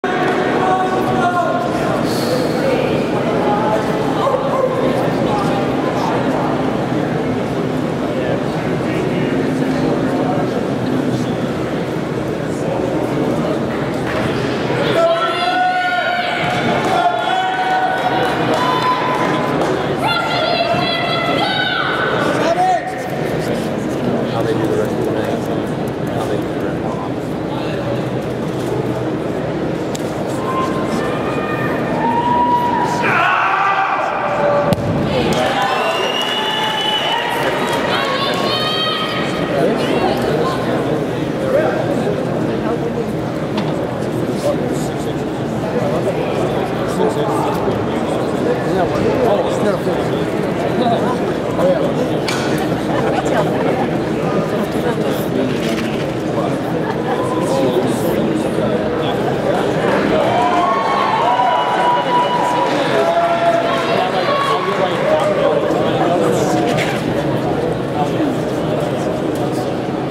Ethan Waldman of Bates College - All American - NCAA D-III Indoor Track & Field Shot Put
Ethan Waldman of Bates College throws his second best shot put toss at 53 feet, 11.75 inches to take 6th place in the Shot Put competion at the NCAA Division III Track & Field Championships in Columbus, Ohio qualifying for All American honors.